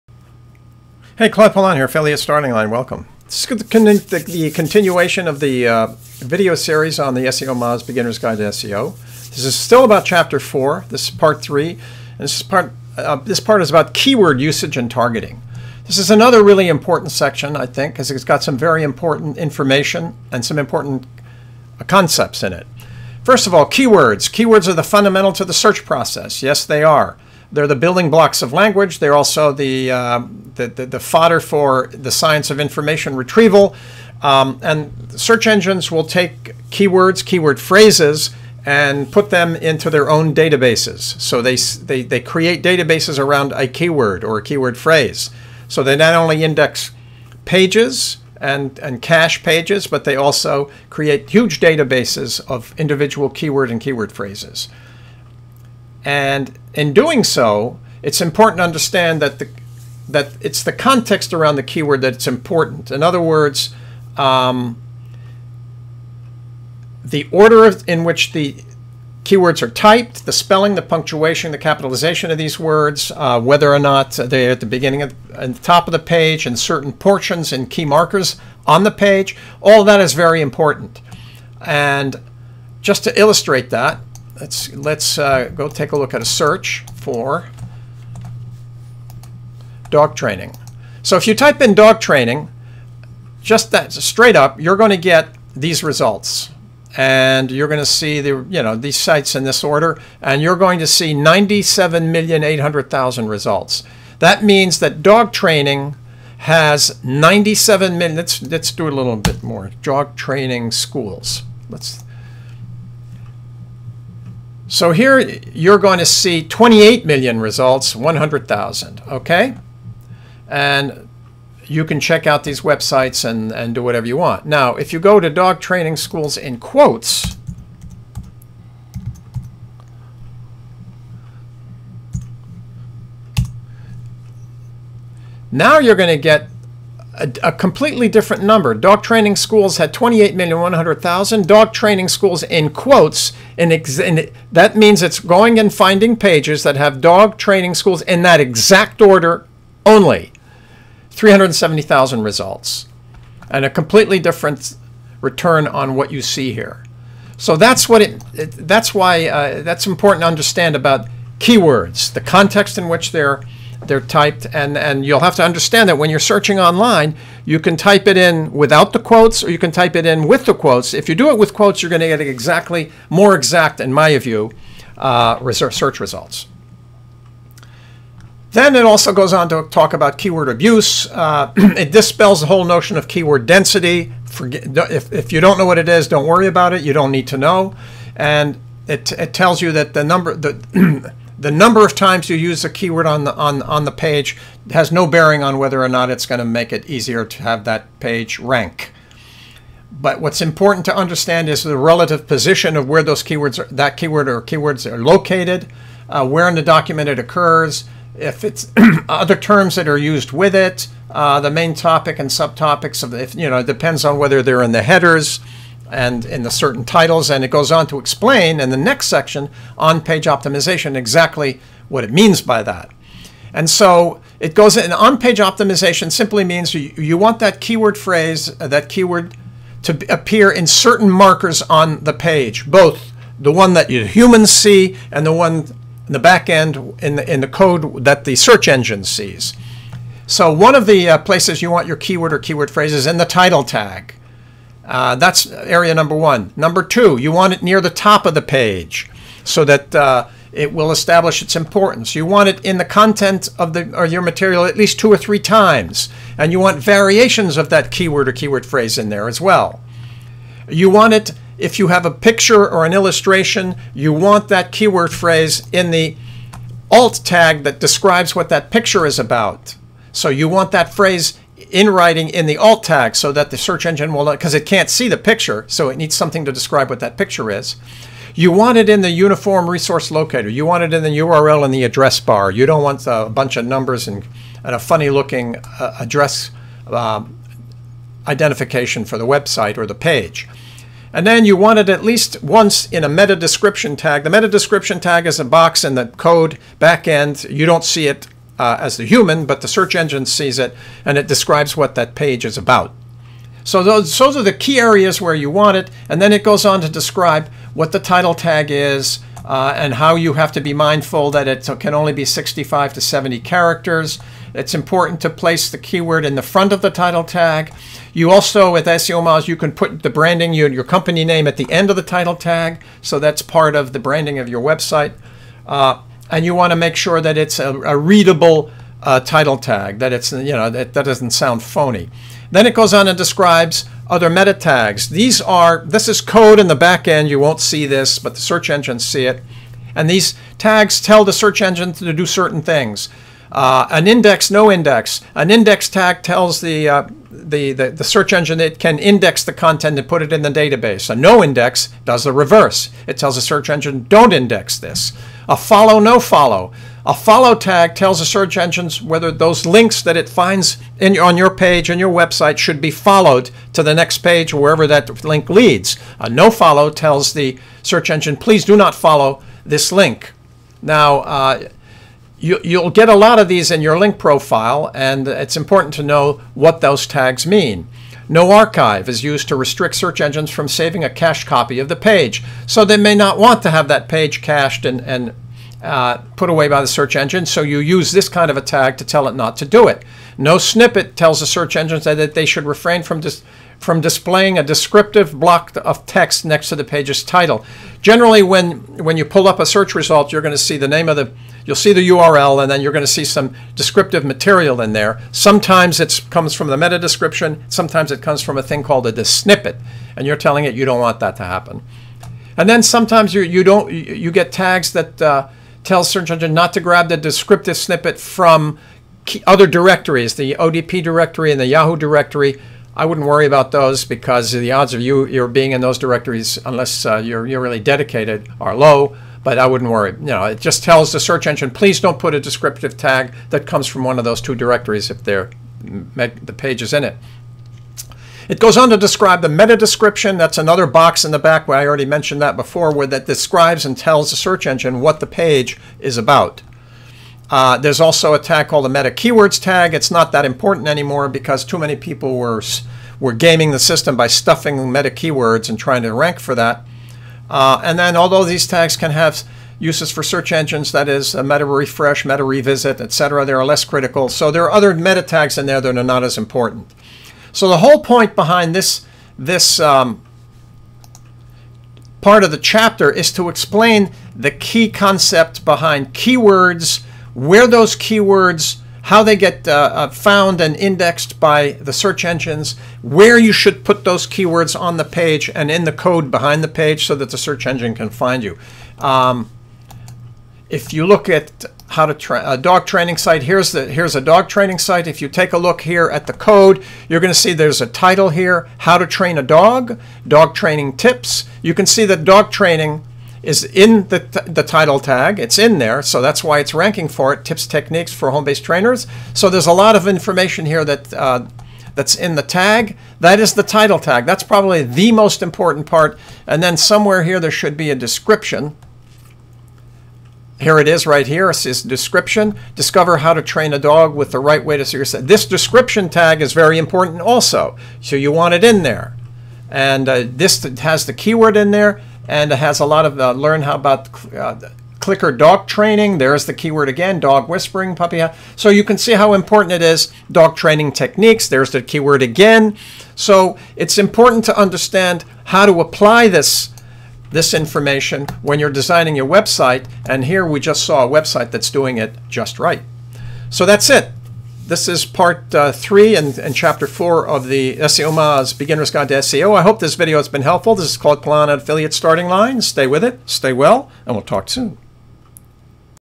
SEOmoz Beginners Guide to SEO -Ch4 Part 3 Review
SEOmoz Beginners Guide to SEO -Ch4 Part 3 Review - Learn how to use keywords and target the right keywords. Learn how search engines use keywords to offer results. You'll find out what is the proper use of keywords and what doesn't work ie keyword density. Learn how to set up On Page Optimization.